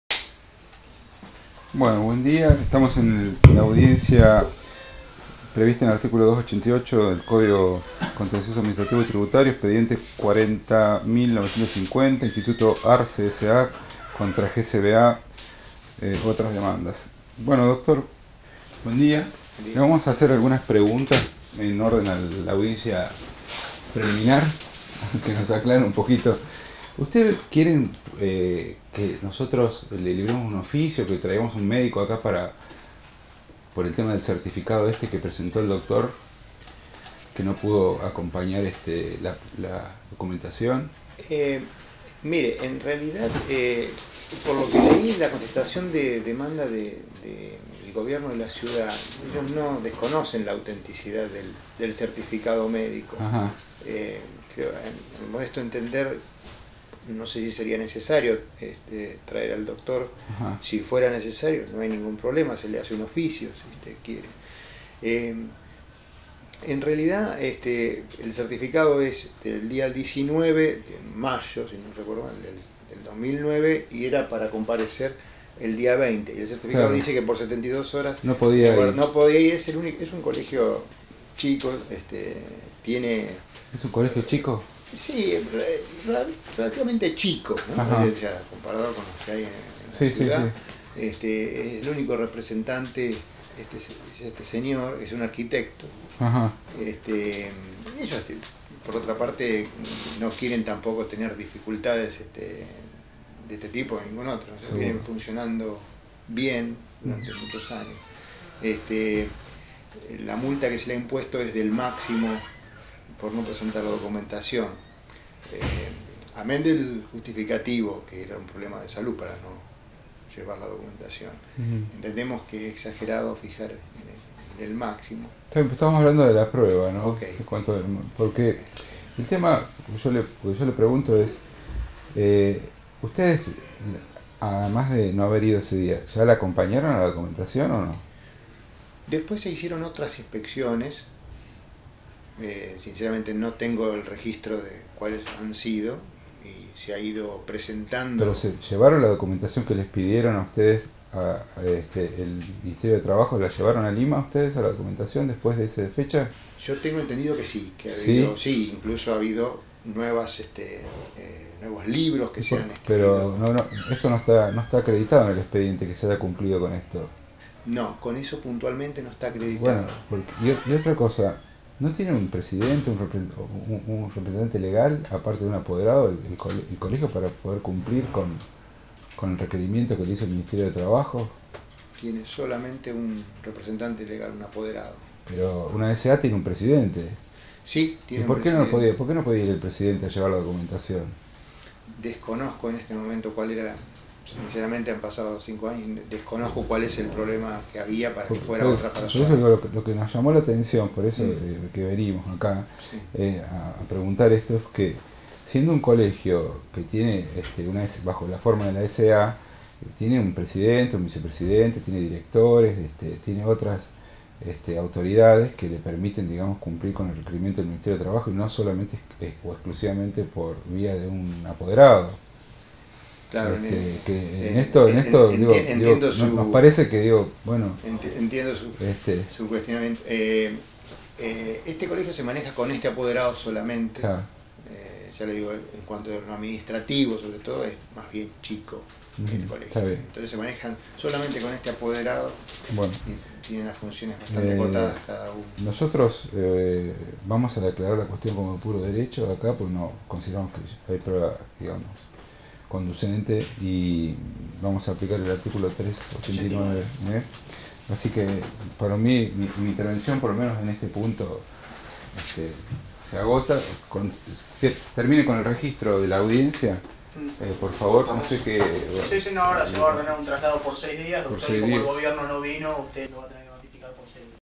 INSTITUTO DE ARCE SA contra GCBA sobre OTRAS DEMANDAS CONTRA LA AUTORIDAD ADMINISTRATIVA
Autos: "Instituto de Arce S.A. contra GCBA sobre Otras demandas contra la autoridad administrativa" expte.: 40950/0
Audiencia: 08/10/2014 - Audiencia del art. 288 CCAyT - Se solicitan explicaciones a la parte actora.-